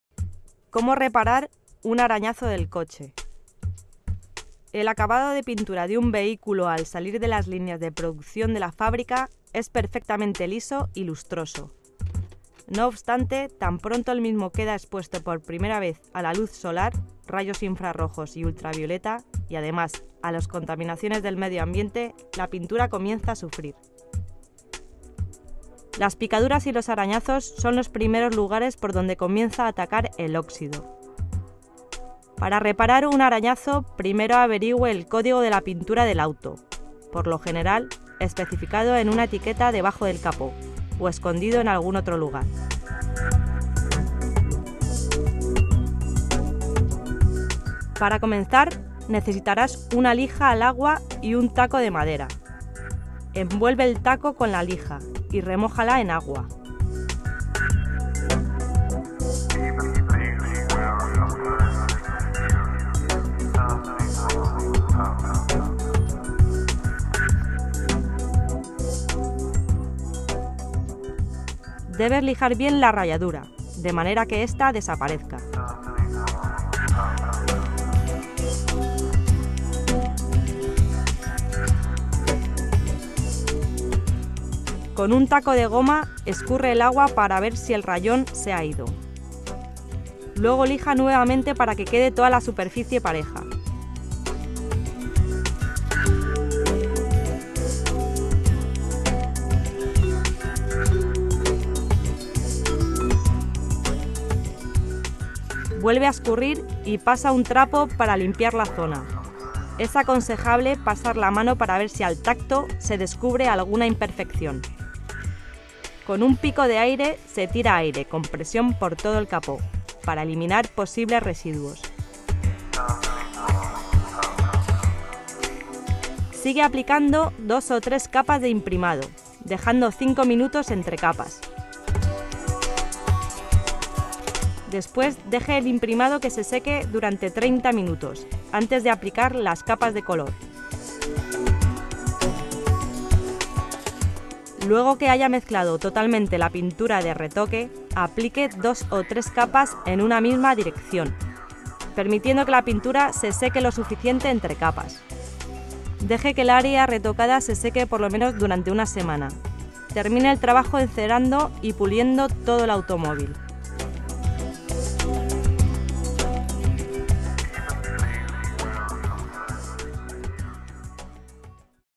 Cómo solucionar un arañazo del coche
Se lija la rayadura y luego se pinta por encima.